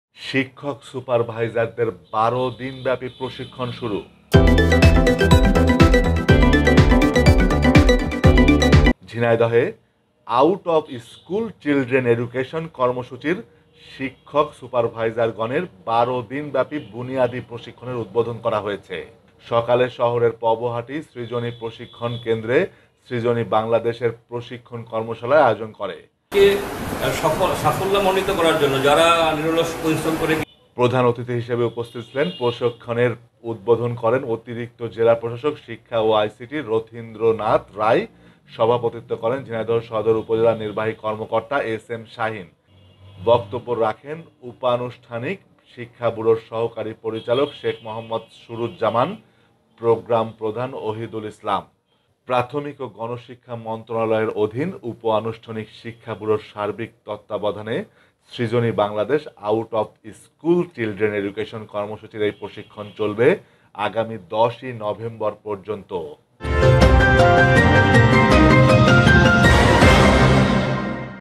ঝিনাইদহে “আউট অব স্কুল চিলড্রেন এডুকেশন কর্মসূচী’র শিক্ষক-সুপারভাইজার প্রশিক্ষন । ঝিনাইদহ টিভি
ঝিনাইদহে “আউট অব স্কুল চিলড্রেন এডুকেশন কর্মসূচী’র শিক্ষক-সুপারভাইজারগনের ১২দিন ব্যাপি বুনিয়াদি প্রশিক্ষনের উদ্ধোধন করা হয়েছে।
রোববার সকালে শহরের পবহাটি সৃজনী প্রশিক্ষণ কেন্দ্রে সৃজনী বাংলাদেশ এ প্রশিক্ষণ কর্মশালার আয়োজন করে। অনুষ্ঠানে। প্রধান অতিথি হিসেবে উপস্থিত থেকে প্রশিক্ষনের উদ্ধোধন করেন অতিরিক্ত জেলা প্রশাসক শিক্ষা ও আইসিটি রথীন্দ্র নাথ রায়। সভাপতিত্ব করেন ঝিনাইদহ সদর উপজেলা নির্বাহী কর্মকর্তা এসএম শাহীন। প্রাথমিক ও গণশিক্ষা মন্ত্রনালয়ের অধীন উপানুষ্ঠানিক শিক্ষা ব্যুরোর সার্বিক তত্ত¡বধায়নে সৃজনী বাংলাদেশ “আউট অব স্কুল চিলড্রেন এডুকেশন কর্মসূচী”র এই প্রশিক্ষণ চলবে আগামী ১০ নভেম্বর পর্যন্ত।